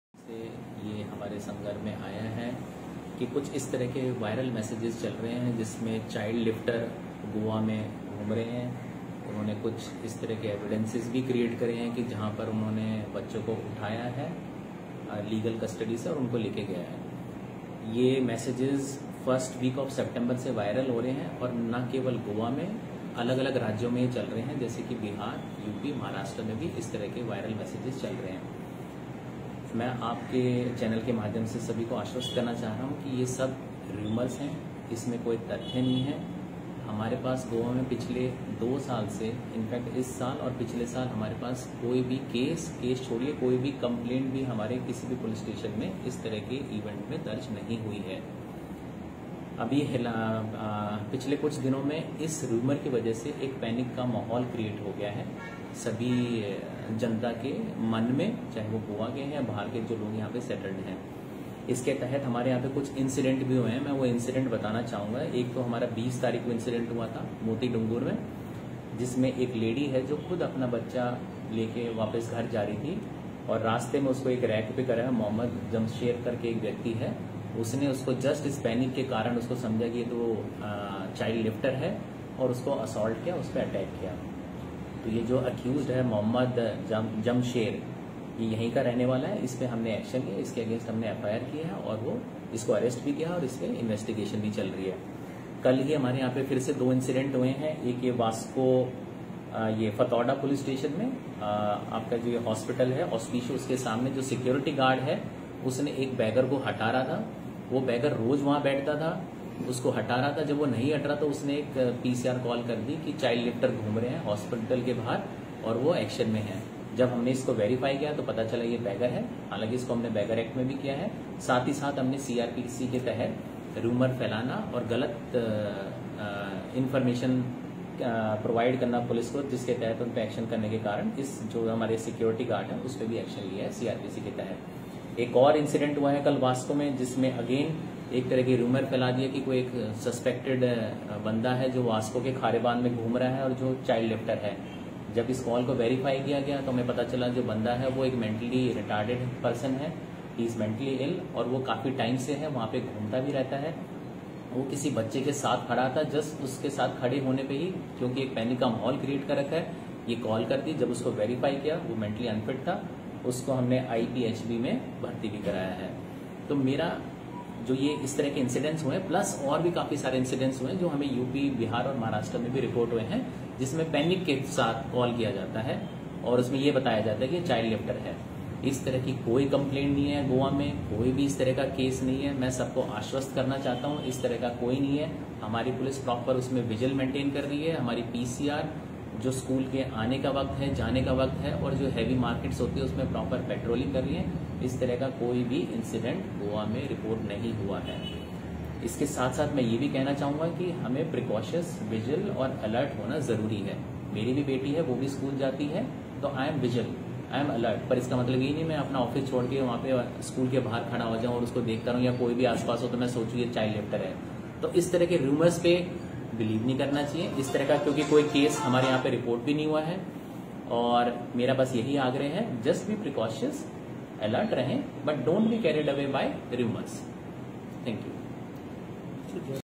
SP South Goa shri Abhishek Dhaniya IPS Briefing the General Public Regarding facts and Rumour
SP South Goa shri Abhishek Dhaniya IPS Briefing the General Public Regarding facts and Rumour and mongering taking place that child lifters are Moving in Goa especially South Goa